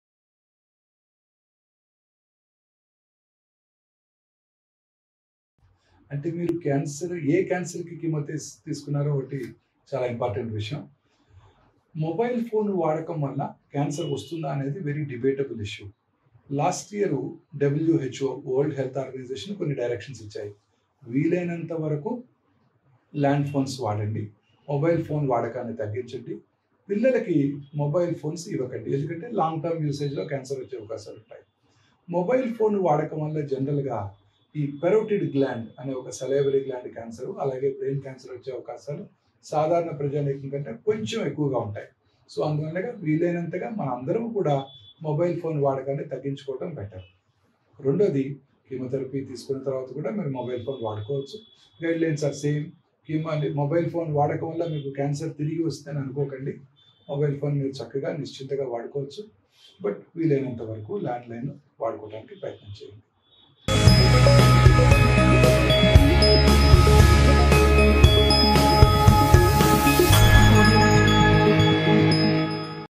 క్యాన్సర్ చికిత్స తీసుకున్న వాళ్ళు మొబైల్ ఫోన్స్ వాడొచ్చా? | CANCER AWARENESS - 6
నేను 6నెలల క్రితం కీమోతెరపీ ట్రీట్మెంట్ తీసుకున్నాను. మొబైల్ వాడకం వలన క్యాన్సర్ వస్తది అని తెలుసు , కీమోతెరపీ తీసుకున్న వాళ్ళు మళ్ళీ మొబైల్ ఫోన్ వాడొచ్చా?

క్యాన్సర్ పై మీకున్న అనుమానాలు, సందేహాలు ఈ పోస్ట్ కింద కామెంట్ చెయ్యండి . - 𝗗𝗿. 𝗠𝗼𝗵𝗮𝗻𝗮 𝗩𝗮𝗺𝘀𝘆, 𝗢𝗺𝗲𝗴𝗮 𝗛𝗼𝘀𝗽𝗶𝘁𝗮𝗹𝘀.

Lets Share- Lets Fight Against Cancer